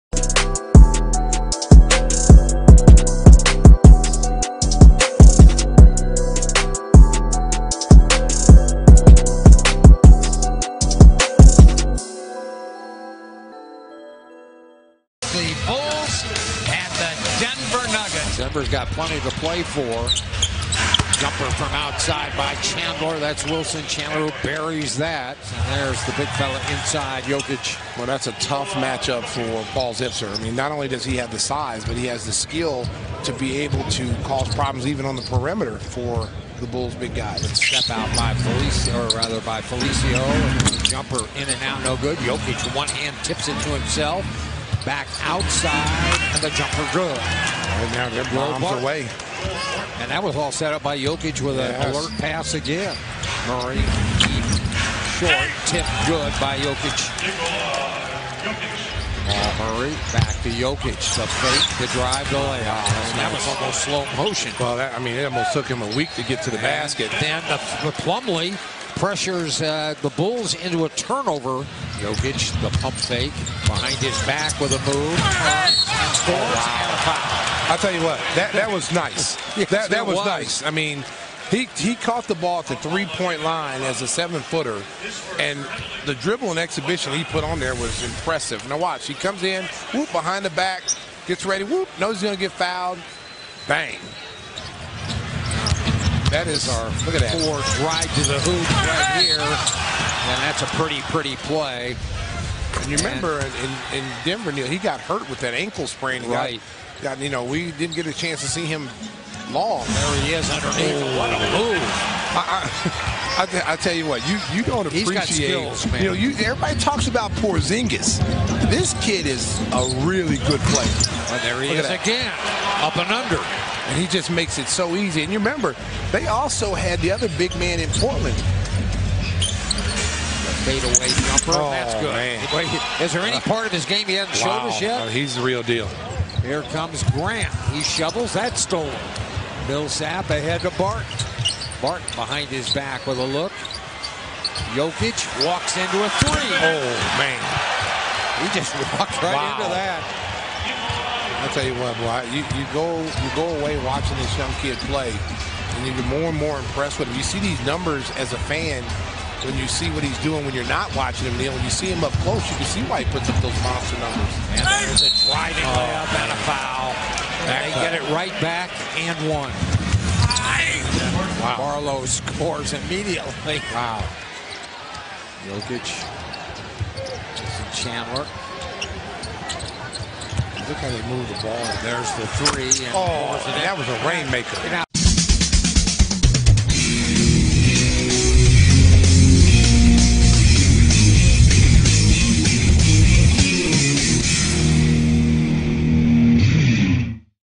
Nikola Jokic vs Bulls Full Highlights (21PTS 7REB 5AST) March 21, 2018
Nikola Jokic vs Bulls Full Highlights (21PTS 7REB 5AST 23MIN) March 21, 2018

Nikola Jokic Drives to the Hoop | Pump-Fake, Behind-the-Back | Dribbling Course

Commentators Compare Jokic with Porzingis | Two SICK Circus Layups

Nuggets Coach Mike Malone Speaks Serbian: "Nikola je nas najbolji igrač! Dobro?"

Denver Nuggets 135:102 Chicago Bulls 2017/18 NBA Season